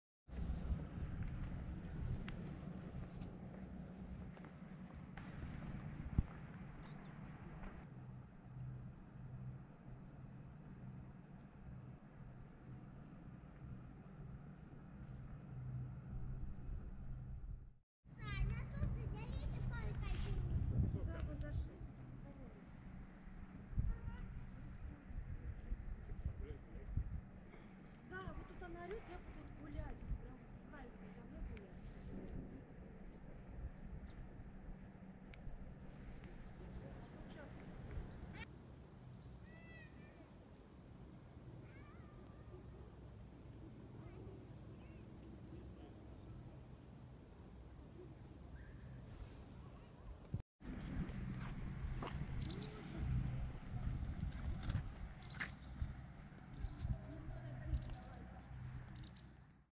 Increíble encuentro con un Zorro a menos de un metro
Encuentro con un zorrino a menos de un metro